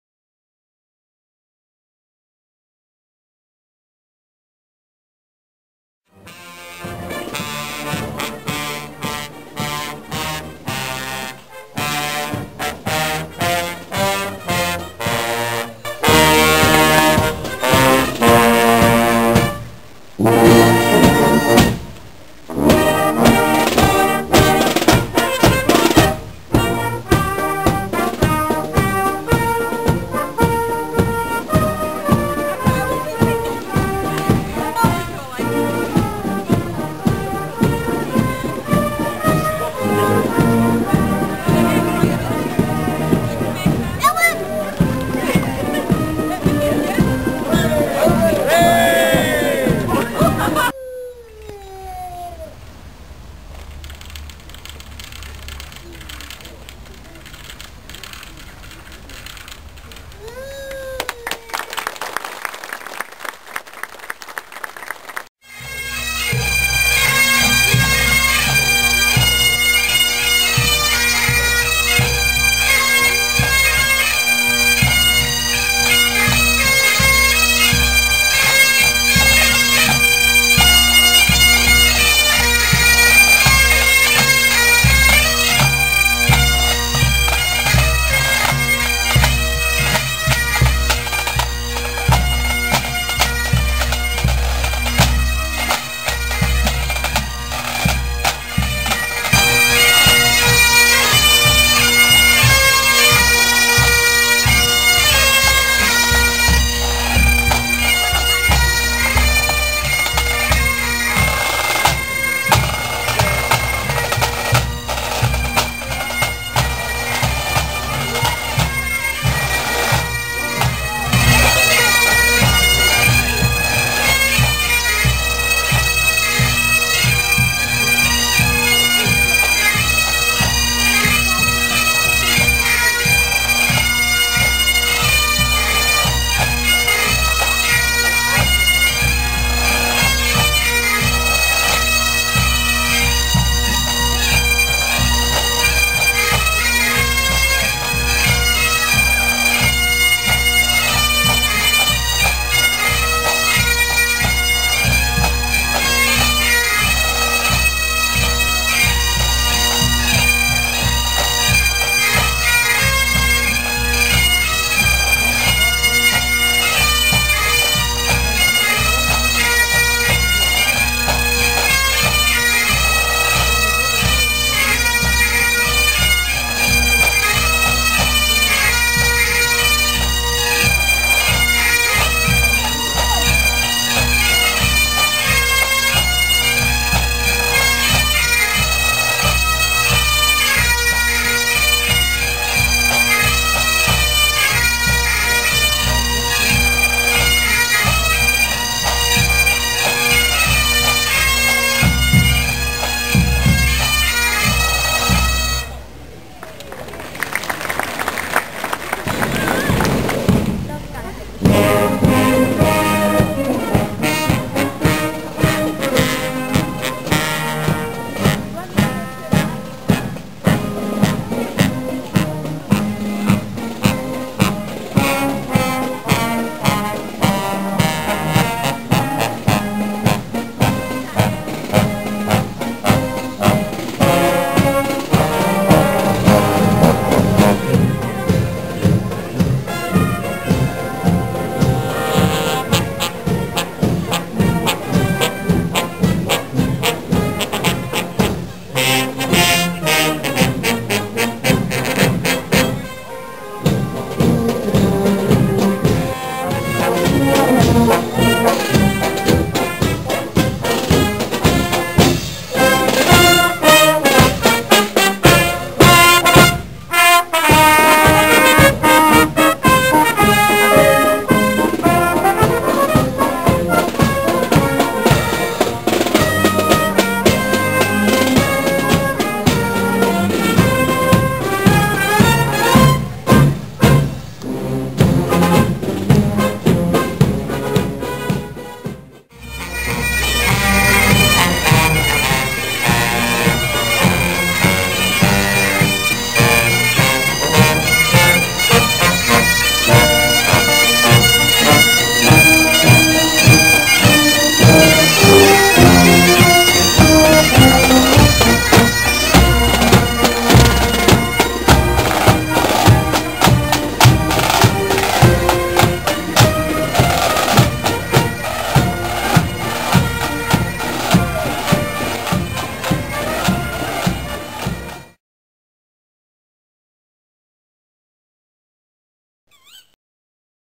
Bagpipe and Silver Bands, Innerleithen 2007
Bands playing during games week, Innerleithen, Scottish Borders, July 2007